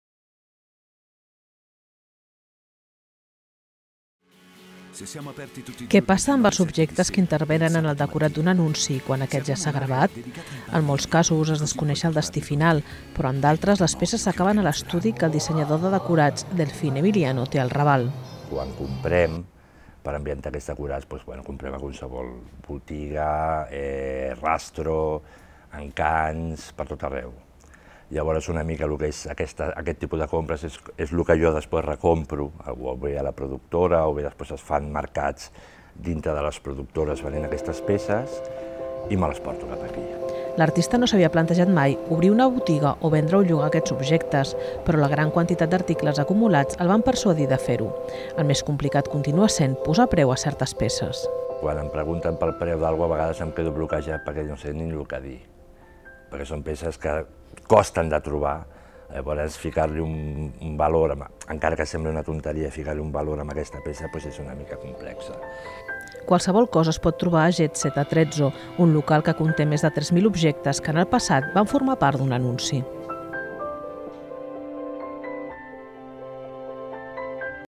La botiga de peces "protagonistes" d'anuncis
En la gravació d’anuncis s'utilitzen decorats per fer més atractiu l’objecte que s’ofereix. A banda de les localitzacions físiques, en aquestes filmacions també intervenen molts articles. Però què passa amb ells una vegada s’acaba la filmació? Algunes d’aquestes peces es troben a un local ubicat al Raval, al Jet Set Atrezzo, propietat del dissenyador de decorats, Delfín Emiliano. Més informació